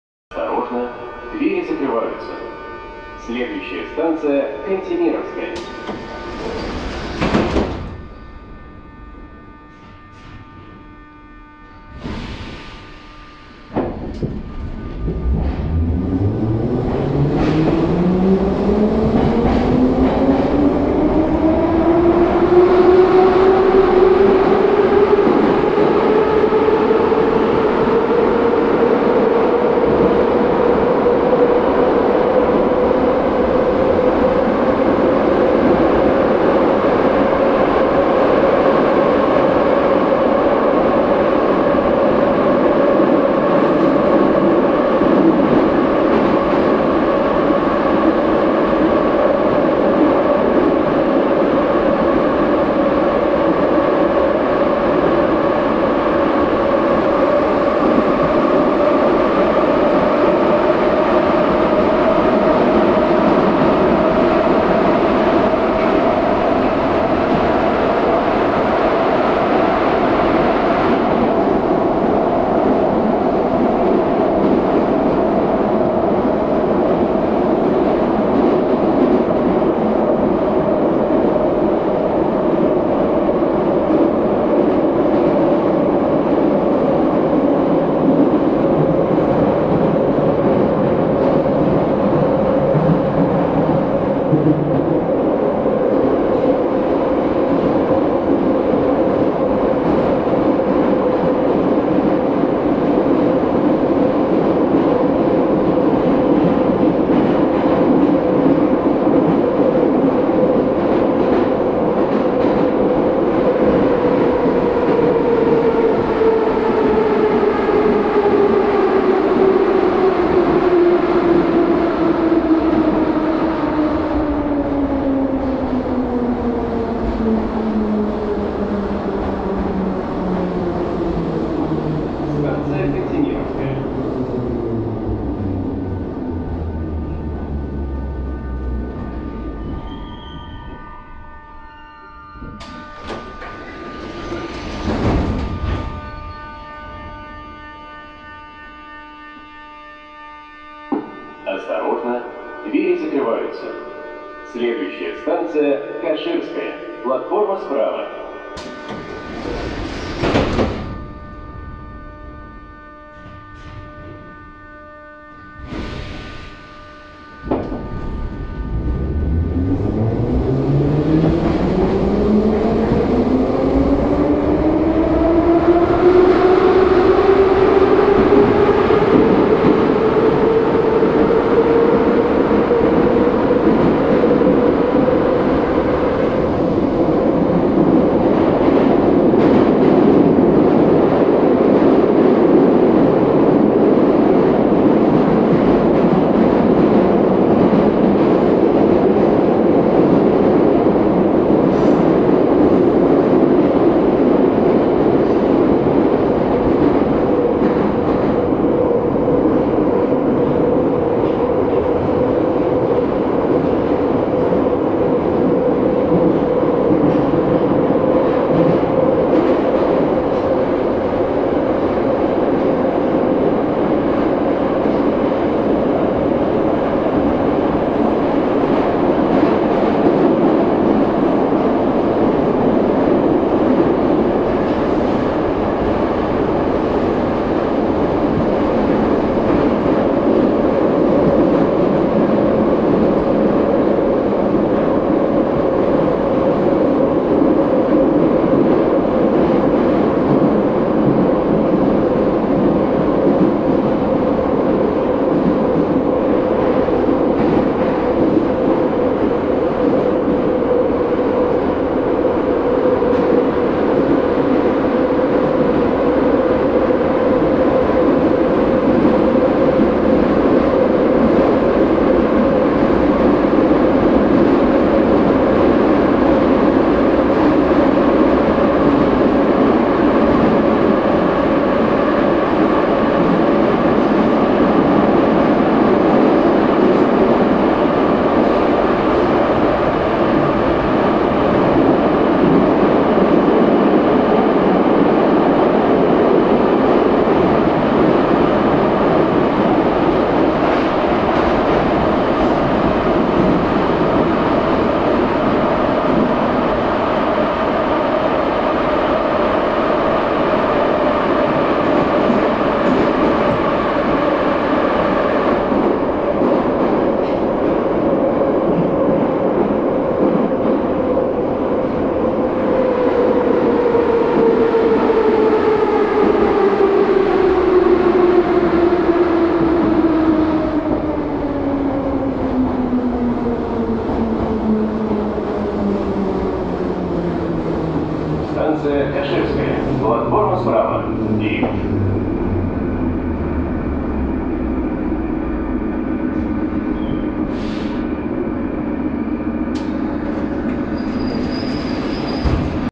Царицыно - Каширская
Пустой вагон "Номерного" на (2) Замоскворецкой линии московского метро.